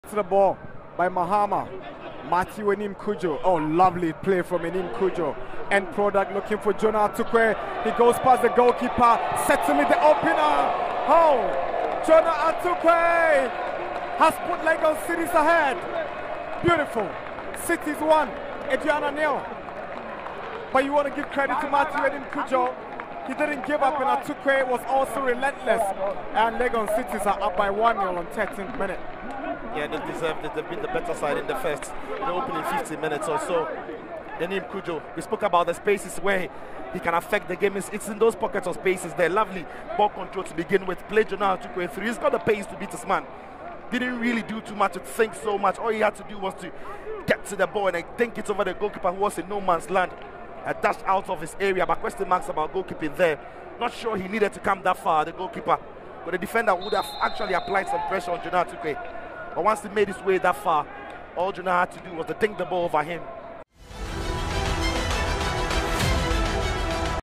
LEGON CITIES VRS ADUANA STARS - Jonah Attuquaye scores for Legon Cities FC in first half.
Goal Highlights:  Jonah Attuquaye scores for Legon Cities FC against Aduana Stars FC in first half.